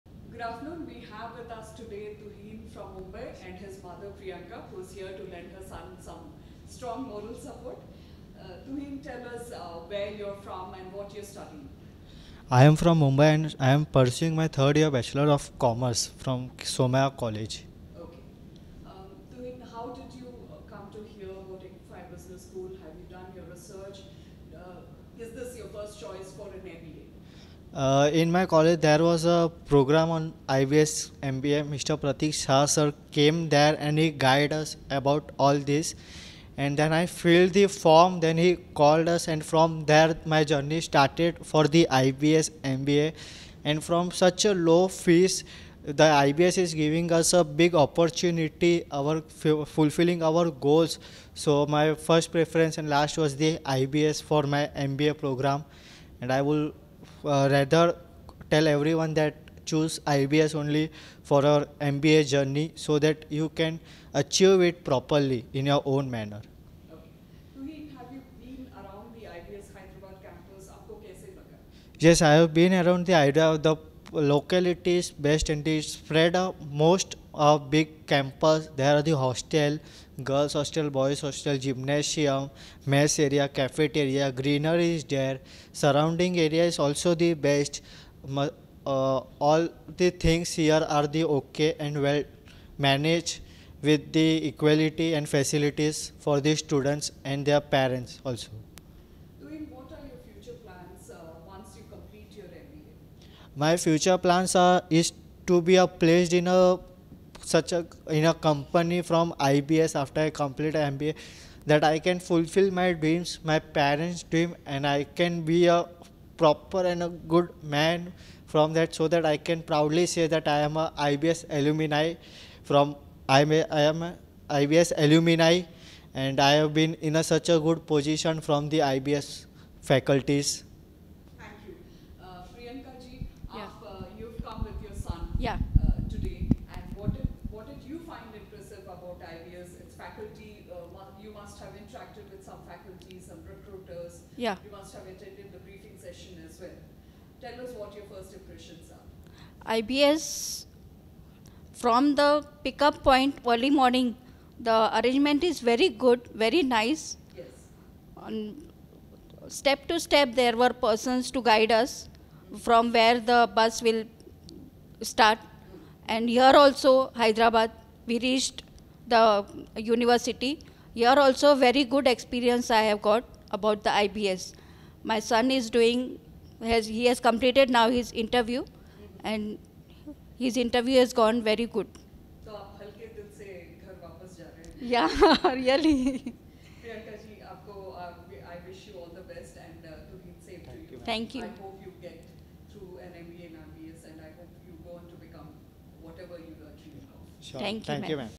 Candidate shared his experience upon completing the IBS Selection process 2024
Candidate for IBS, provided an in-depth overview of his experience during the IBS selection process in 2024. His enlightening insights offered valuable perspectives on the journey candidates undertake to become part of the IBS community.